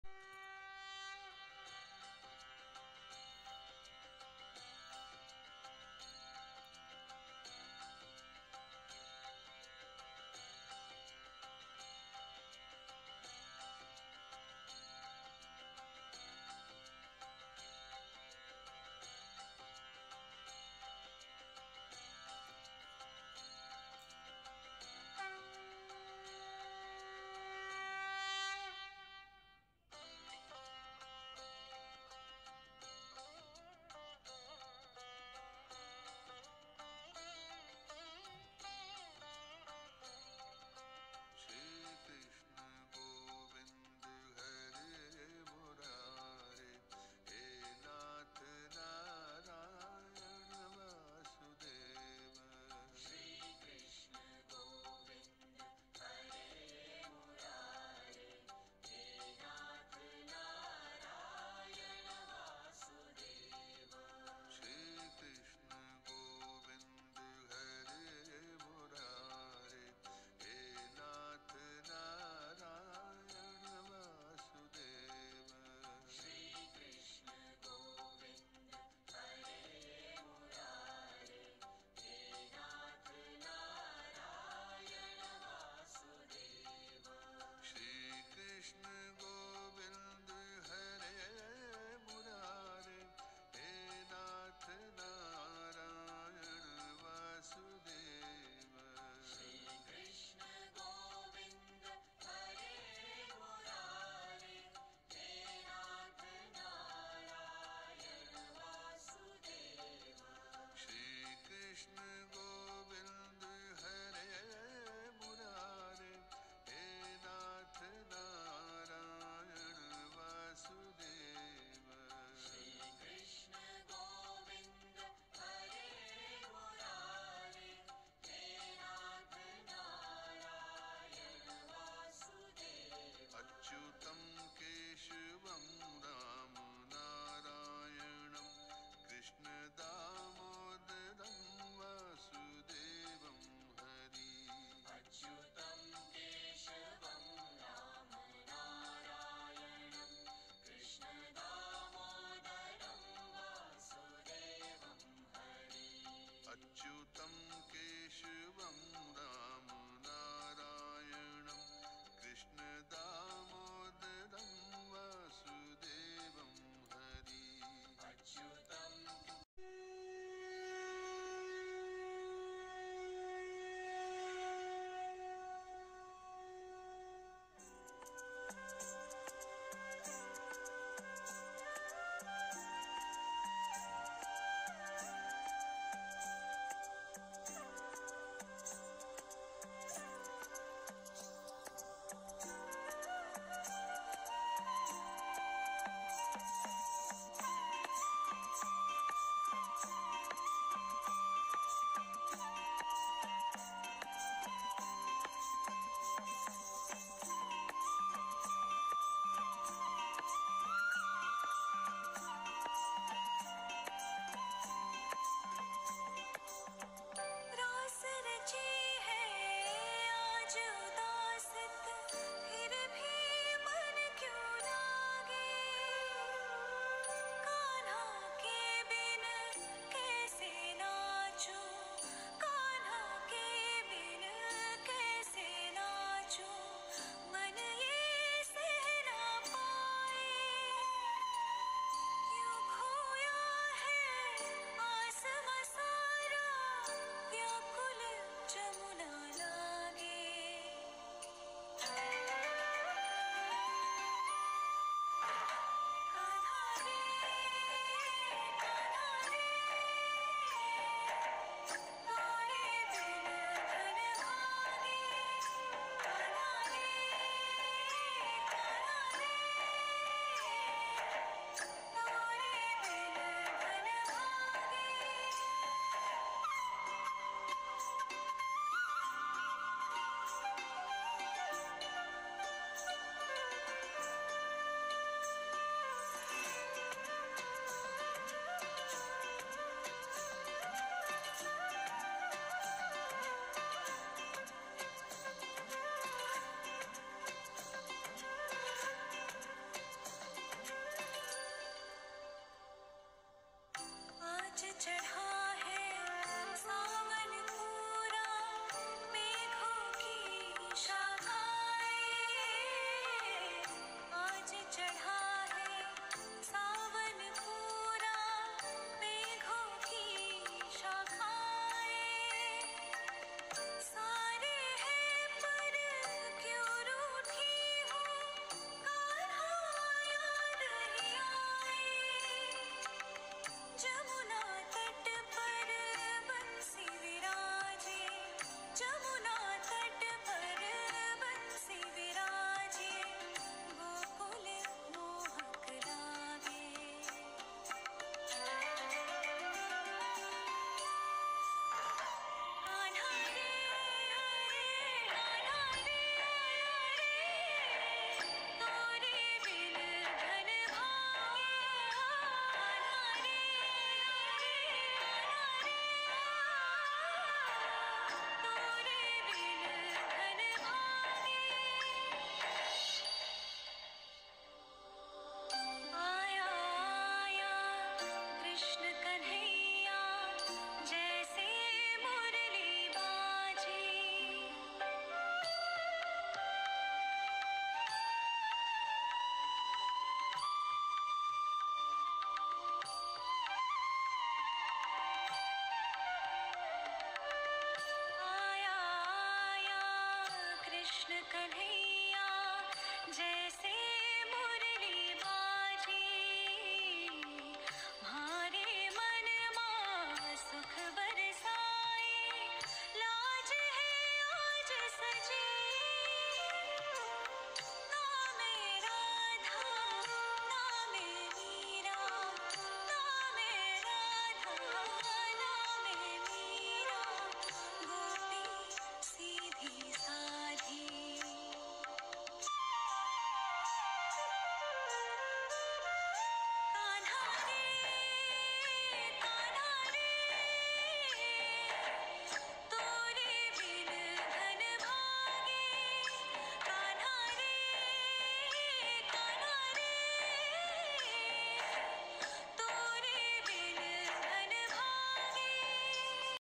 Janmashtami celebrations in our home Bahrain ||sambalpuri family
Janmashtami ra ବହୁତ୍ ଅଭିନନ୍ଦନ। This video is all about the janmashtami celebration at our home in Bahrain. we stay in outside  India but we never forget our culture n traditional value. we celebrate all the Indian festival here in Bahrain.